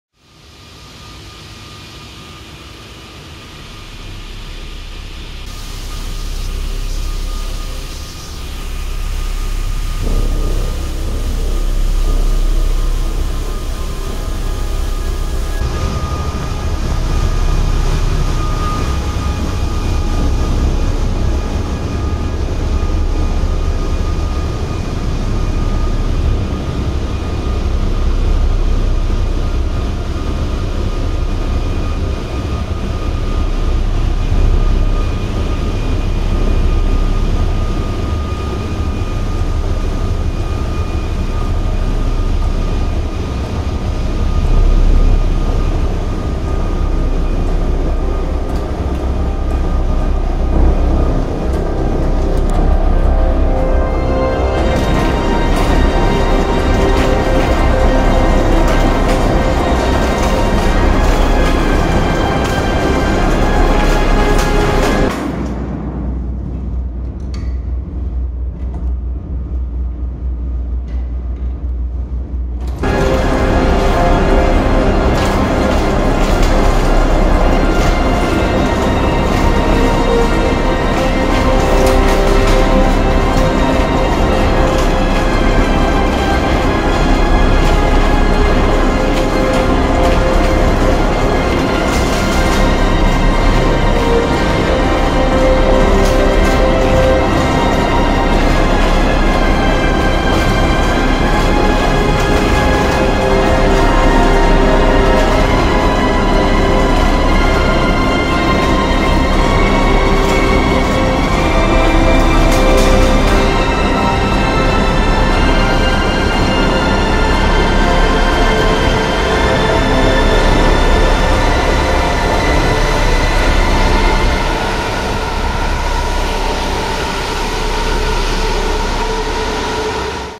The secret container ship rail ride
The pipe duct is a long... well, duct, that runs along the bottom of the ship. Not all the way but still pretty long. It has to be regularly checked for presence of leaks and the float switch alarms (activated by flooding) placed on each end of the duct have to be tested. And it would be a massive pain to crawl all this way... so we have a neat little cart there.